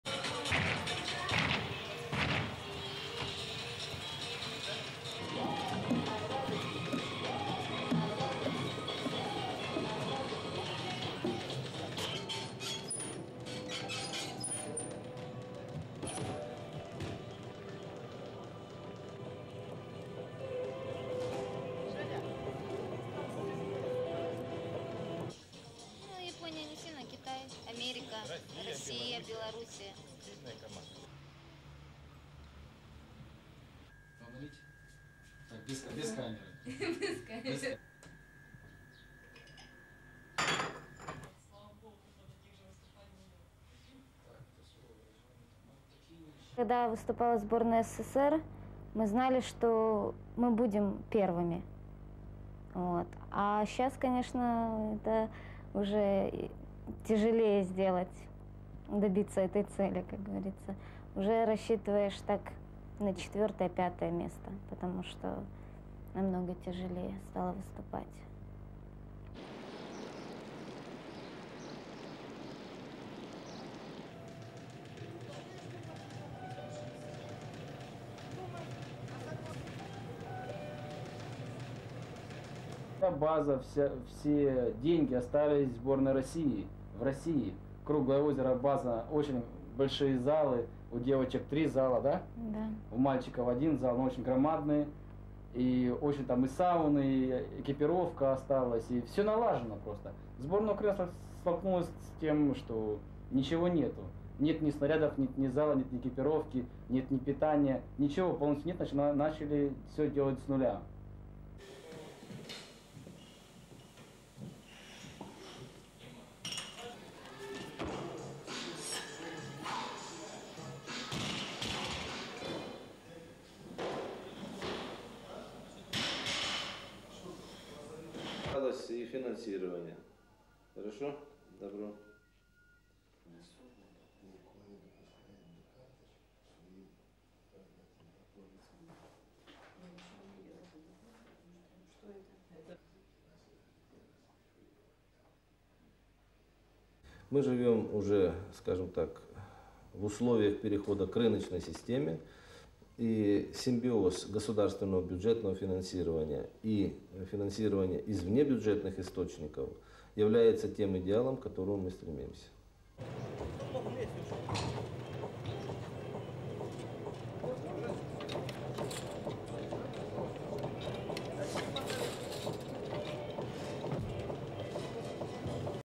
UKRAINE: UKRAINIAN OLYMPIC TEAMS FACE FINANCIAL HARDSHIPS
(13 Jul 1996) Russian/Nat

Ukrainian sports stars were once considered among the cream of the athletics world,  winning countless medals and world records for the Soviet Union.

No expense was spared on preparing them for international competitions.

But the break-up of the Soviet Union has brought hard times and the Ukrainian  Republic's first independent team has barely enough money to get to Atlanta for the  Olympic Games.

At the Ukrainian Olympic training base just outside Kiev, gymnasts are going through  their paces for the Atlanta Games.

This will be the first year that these sportsmen will perform at the Olympics under the  Ukrainian flag - at the Barcelona games they were still part of a C-I-S (Commonwealth  of Independent States) team.

But the demise of the joint team also meant the end of joint funding, bringing many  highflying Ukrainian sportsmen and women down to earth with a bump.

Despite the recent financial hardships, 25-year-old gymnast Vladimir Shamenko won a  bronze at the 1994 World Championships and is heading to Atlanta.

His wife Ludmilla, a European gold medalist, has to sit out this year's games because  she's pregnant with their first child.

In the old days, the couple would have been given special privileges and  accommodation - today they share a one-room apartment far from the training centre.

They live like any other Ukrainian couple -- suffering the rigours of sky-high inflation  which make buying special foods required for Vladimir's training very difficult.

Only their sitting room - which doubles as a trophy cabinet - picks them out from the  crowd.

Although the Shamenkos wouldn't want the U-S-S-R to return, the young couple do  miss the sense of belonging to an Eastern bloc powerhouse.

SOUNDBITE: (Russian)
When we took part in the Soviet team we knew that we would be first. But now of  course it's harder to do so - to get those sort of results. Now I usually think that I'll  come fourth or fifth because it's much harder for us to compete.
SUPER CAPTION: Ludmilla Shamenko, Ukrainian gymnast

Kiev's Central stadium is in need of repair, but it will have to wait.

In Soviet times money was poured into training facilities across the communist block.

But it was Russia that was home to the world's largest and best-equipped training  centre -- a legacy that benefits the present-day team, much to the envy of their  Ukrainian neighbours.

SOUNDBITE:
After the break-up of the Soviet Union all the finances and training camps remained in  Russia. Krugloe Ozero - the main training centre - has very large gyms. There are three  gyms just for the girls and one for the guys. It's an enormous sports hall, there's a  sauna, they've got all the sports gear and everything is in good order. But the Ukrainian  team has nothing. No equipment, no sports hall and no sports clothes. No food. There's  nothing so we had to start again from scratch.
SUPER CAPTION: Vladimir Shamenko, Ukrainian Olympic gymnast

For those pumping iron in the weight room the Atlanta Olympics' poster on the wall is  an incentive to go for gold.

But even for European, Olympic and World champion weightlifter Timur Raimozov this  goal until recently seemed a distant one.

The hard-pressed government couldn't afford to send the whole team to the United  States.

It was also unable to invest in world standard equipment which deterred much needed  foreign competition from coming to Ukraine.

Valery Borzov is head of Ukraine's Olympic committee and a national hero.

SOUNDBITE:
SUPER CAPTION: Valery Borzov, Chairman Ukraine Olympic Committee